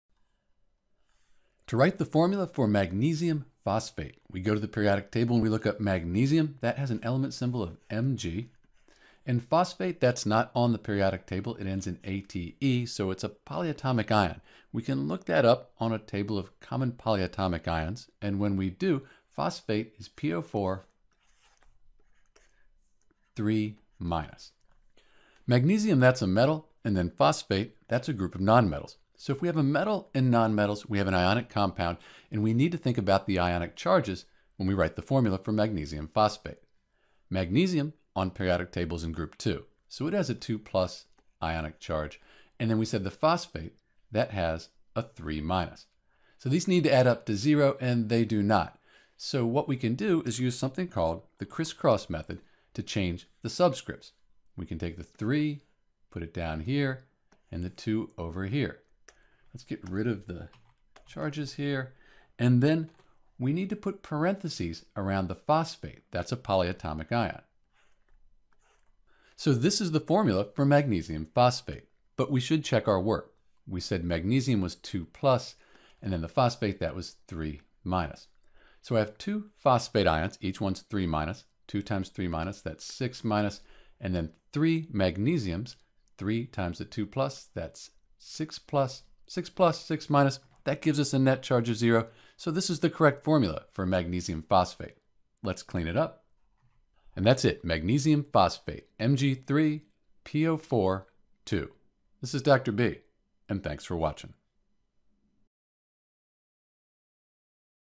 How to Write the Formula for Magnesium phosphate
In this video we'll write the correct formula for Magnesium phosphate, Mg3(PO4)2.

To write the formula for Magnesium phosphate we'll use the Periodic Table, a Common Ion Table, and follow some simple rules.

Because Magnesium phosphate has a polyatomic ion (the group of non-metals after the metal) we'll need to use a table of names for common polyatomic ions, in addition to the Periodic Table.

Keys for Writing Formulas for Ternary Ionic Compounds:

1. Write the element symbol for the metal and its charge using Periodic Table.
2. Find the name and charge of the polyatomic ion on the Common Ion Table.
3. See if the charges are balanced (if they are you're done!)
4. Add subscripts (if necessary) so the charge for the entire compound is zero.
5. Use the crisscross method to check your work.

For a complete tutorial on naming and formula writing for compounds, like Magnesium phosphate and more, visit: